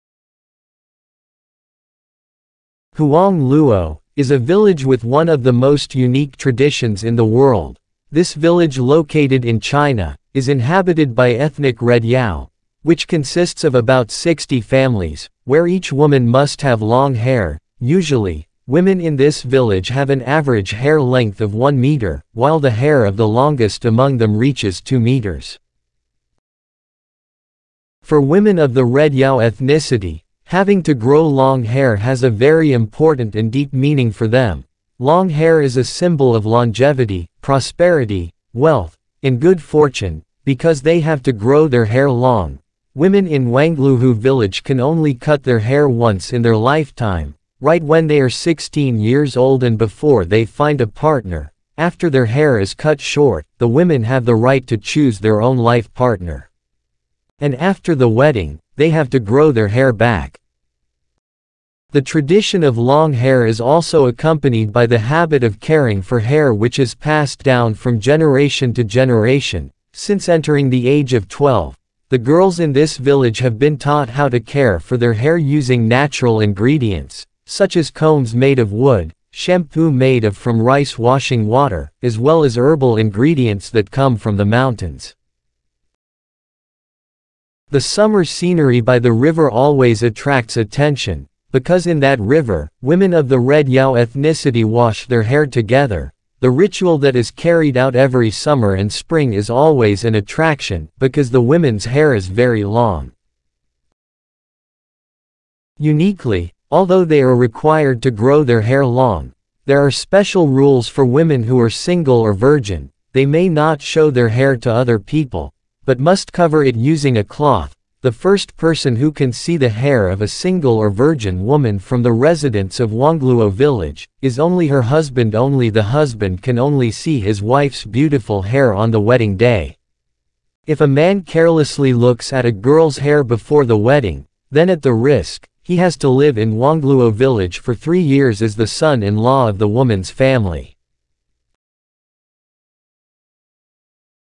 China Huangluo Yao - All Women in This Village Must Have Long Hair
Huangluo Yao is one of the unique villages in China, because this village requires every woman to have long hair. The women of Huangluo Yao Village have long hair that measures one meter on average, and the longest among them reaches 1.9 meters. For women of the Red Yao ethnicity, having long hair is not just a meaningless culture. More than that for them long hair is a symbol that symbolizes longevity, prosperity, etc. So that the people in this village are known as the people with the longest hair in the world. And thanks to this culture, they managed to break the record for the longest hair in the world.